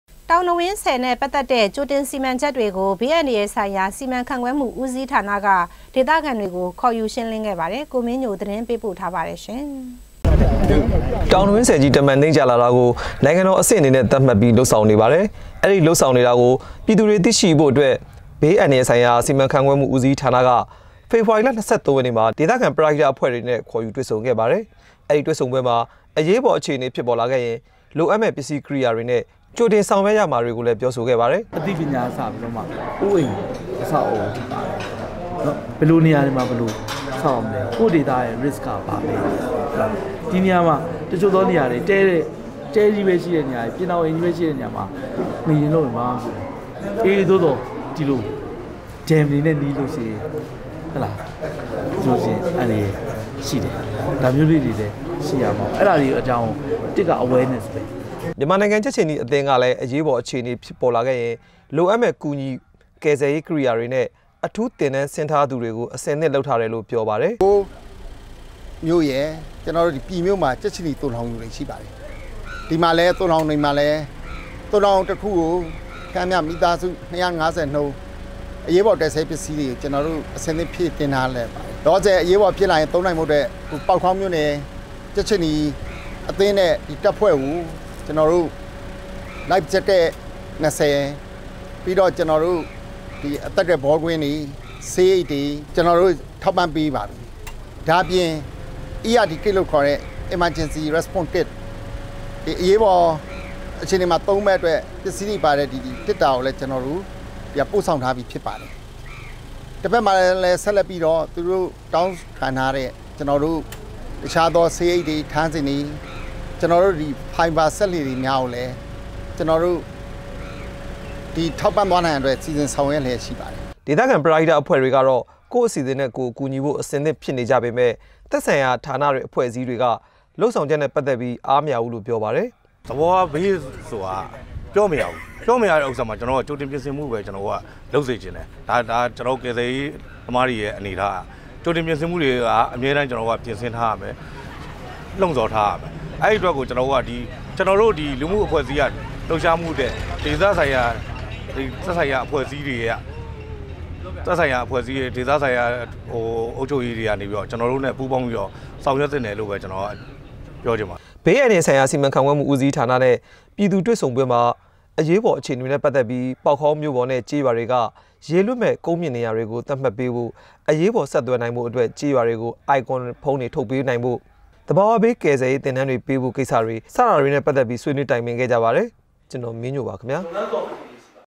DVB - ေတာင္န၀င္းဆည္ ၾကဳိတင္စီမံခ်က္ေတြကို ေဘးအႏၱရာယ္ဆိုင္ရာ စီမံခန္႔ခြဲမႈ ရွင္းလင္း
ေတာင္န၀င္းဆည္နဲ႔ ပတ္သက္တဲ့ ၾကိဳတင္စီမံခ်က္ေတြကို ေဘးအႏၱရယ္ဆိုင္ရာ စီမံခန္႔ခြဲမႈဦးစီး႒ာန ကေဒသခံေတြကိုေခၚယူ ရွင္းလင္းခဲ႔ပါတယ္။ ဒီသတင္းကို ကိုမင္းညိဳ ကေပးပို႔ထားတာပါ